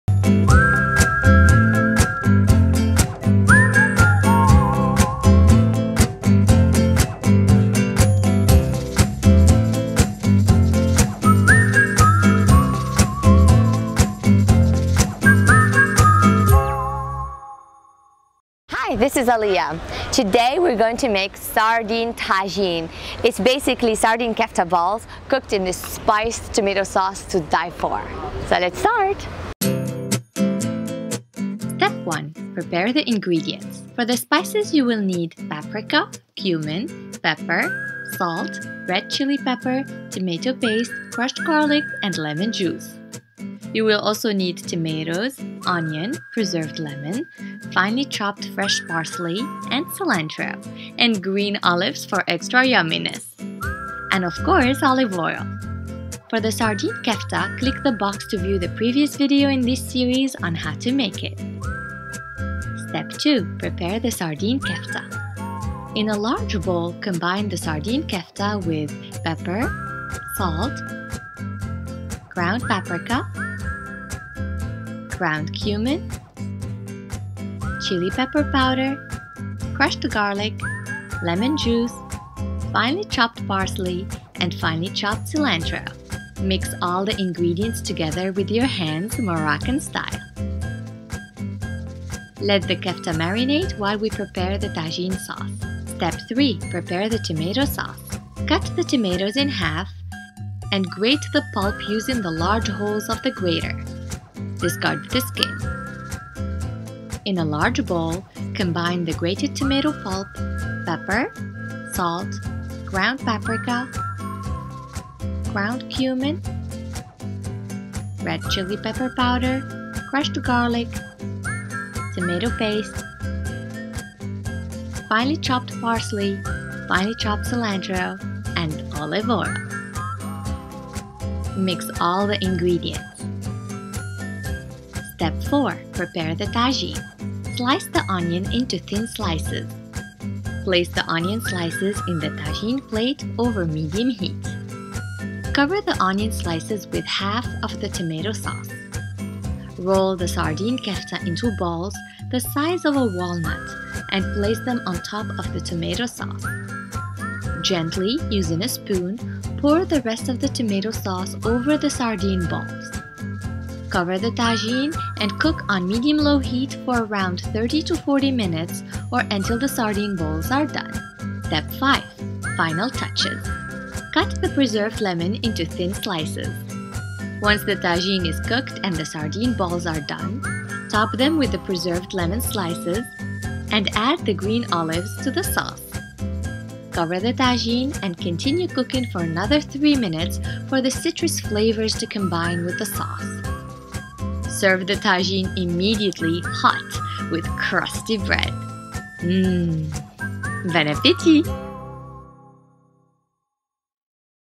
[ENG] Tagine Sardine Kefta / طاجين كفتة السردين - CookingWithAlia - Episode 447
This tagine of sardine kafta is a Moroccan favorite during summer days!

INGREDIENTS:
Sardine Kefta Ingredients:
1 kilo sardine kefta
2 garlic cloves, crushed
1 teaspoon ground paprika
1 teaspoon ground cumin
1/2 teaspoon red chilli pepper powder (to taste)
Salt and pepper (to taste)
1 tablespoon fresh lemon juice
1 tablespoon finely chopped parsley
1 tablespoon finely chopped cilantro

Ingredients for the Tomato Base:
3 tomatoes, grated
3 garlic cloves, crushed
1 tablespoon finely chopped parsley
1 tablespoon finely chopped cilantro
2 tablespoons canned tomato paste
2 teaspoons ground paprika
2 teaspoons ground cumin
1/2 teaspoon red chilli pepper powder (to taste)
3 tablespoons olive oil

For decoration :
1/2 preserved lemon
A handful of green olives

RECIPE:
Step 1: Prepare the Sardine Kefta
1- In a large bowl combine the sardine kefta (ground sardines), pepper, salt, ground paprika, ground cumin, chili pepper powder, crushed garlic, lemon juice, finely chopped parsley, and finely chopped cilantro.  Mix all the ingredients together.

Step 2: Prepare the Tomato Sauce
1- Cut the tomatoes in half, and grate the pulp using the large holes of the grater. Discard the skin.
2- In a large bowl, combine the grated tomato pulp, pepper, salt, ground paprika, ground cumin, red chili pepper powder, crushed garlic, tomato paste, finely chopped parsley, finely chopped cilantro, and olive oil. Mix all the ingredients.

Step 3: Prepare the Tagine
1- Slice the onion into thin slices.
2- Place the onion slices in the tagine plate over medium heat.
3- Cover the onions slices with half of the tomato sauce.
4- Roll the sardine kefta into balls the size of a walnut and place them on top of the tomato sauce.
5- Gently, using a spoon, pour the rest of the tomato sauce over the sardine balls.
6- Cover the tagine and cook, on medium-low heat, for around 30-40 minutes or until the sardine balls are done.

1- Cut the preserved lemon into thin slices.
2- Once the tagine is cooked and the sardine balls are done, top them with the preserved lemon slices and add the green olives to the sauce.
3- Cover the tagine and continue cooking for another 3 minutes for the citrus flavors to combine with the sauce.
4- Serve the tagine immediately, hot, with crusty bread.